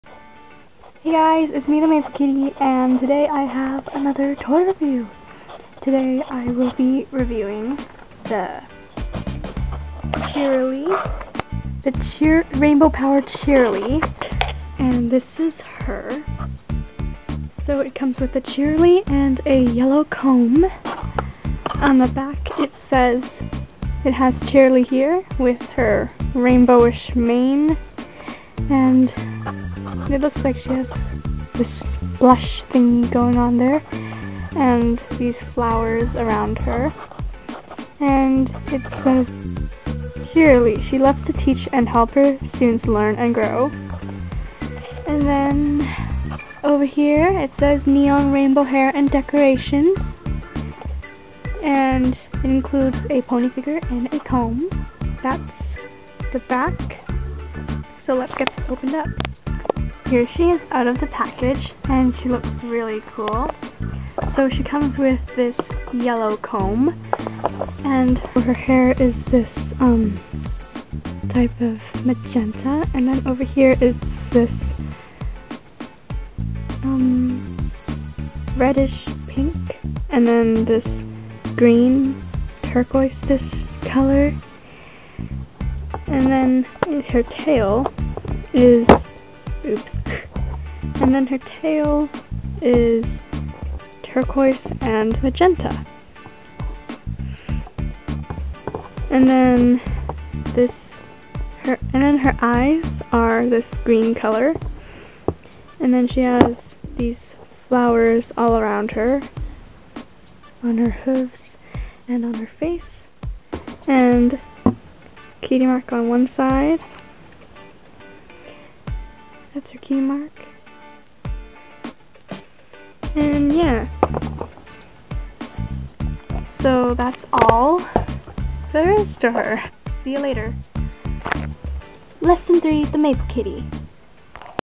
Rainbow Power Cheerilee Toy Review
I know the mane style on the thumbnail looks weird but i had nothing else to make it. This video was filmed a loooong time ago i just never edited it. More videos coming soon! I'll work on it...